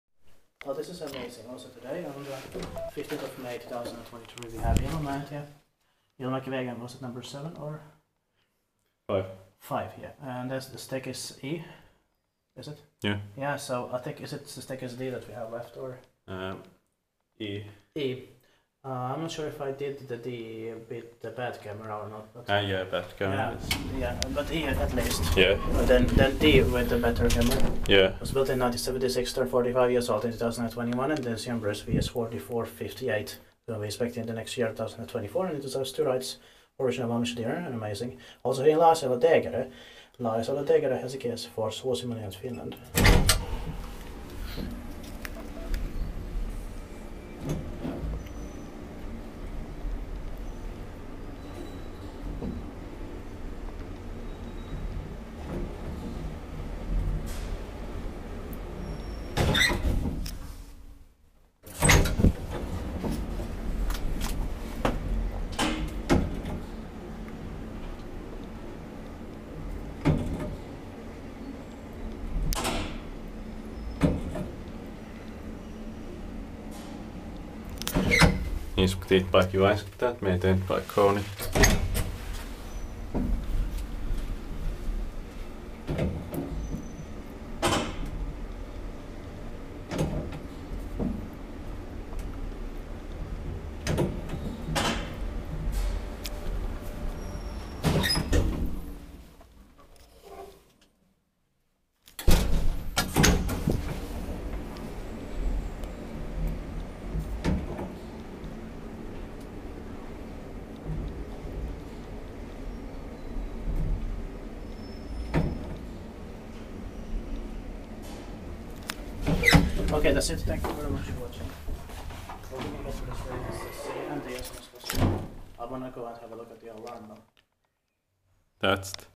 Old ValmetSchlieren traction elevator/lift in Ilomäentie 5E, Laajasalo, Helsinki
Location: Ilomäentie 5E, Laajasalo, Helsinki, Finland
Brand: ValmetSchlieren, Filmed with Elevators from Finland
Year installed/modernized: 1976
Serial number: VS 4458
Floors: 5(K, 1, 2, 3, 4)
Capacity: 4 persons or 325 kg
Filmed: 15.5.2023